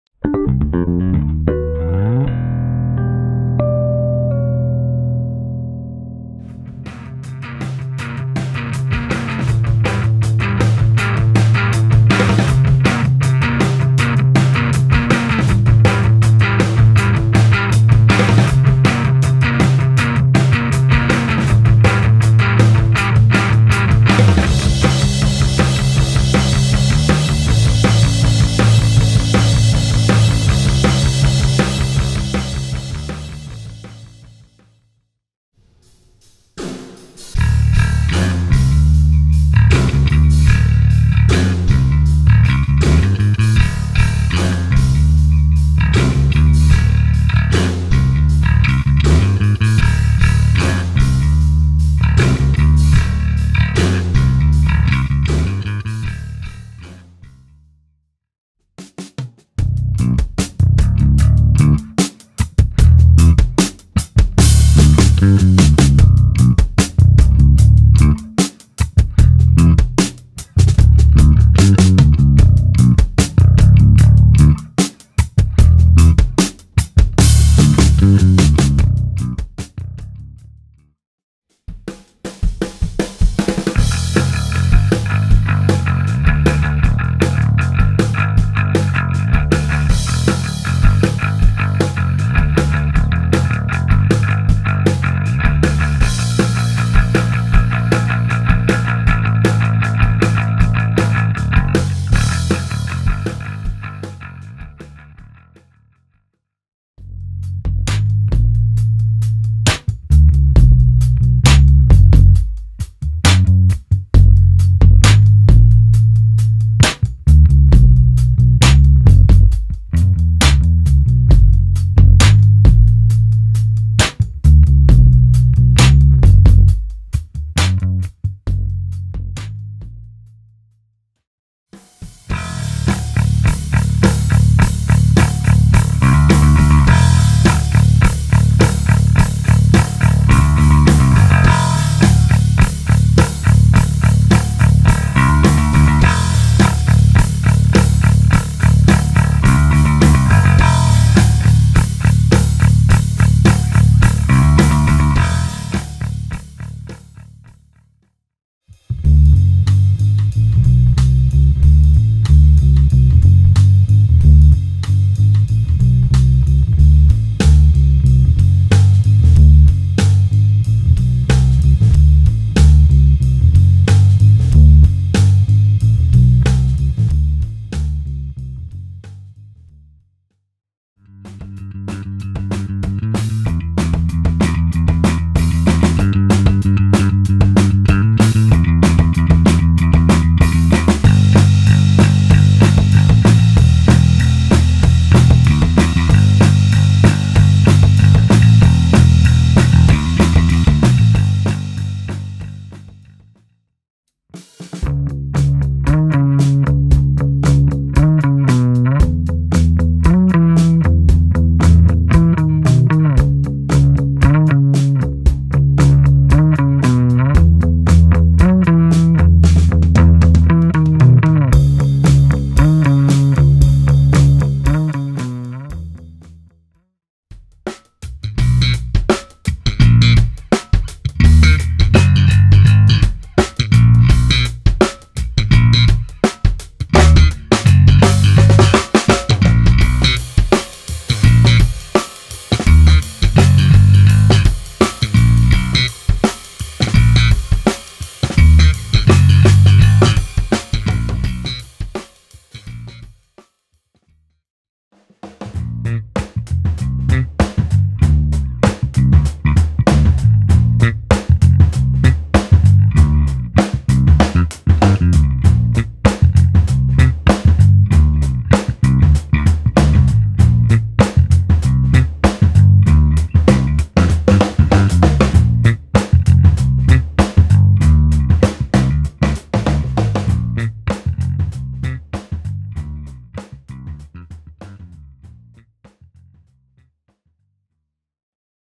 Gallien-Krueger Fusion S Series Heads Demo
We run the Fusion S Series heads through multiple genres, playing styles, and basses to show off its unlimited tone potentials. Please listen with headphones for the best experience and let us know which sound you like best!

The Fusion S Series Heads are GK’s brand new Class D Model heads with a tube preamp. The rich warmth of tubes combined with the quick, punchy power section that GK bass amps are known for is an experience like no other. These high powered heads incorporate three 12ax7 tubes in the preamp, including one dedicated to the overdrive channel, making it GK’s first ever tube driven overdrive channel. Illuminated knobs add an extra level of sophistication and design by not only making it easier to dial in your tone on stage and but adding a beautiful stage presence as well. Each illuminated knob is also push activated, with additional voicing filters and tonal characteristics built in.

Credits:
Recorded in Portland, Oregon at Destination Universe by Victor Nash
Camera Operator: Merav Yami
Edited By: Forest Gallien
Direction and Production: Forest Gallien
Performer: Forest Gallien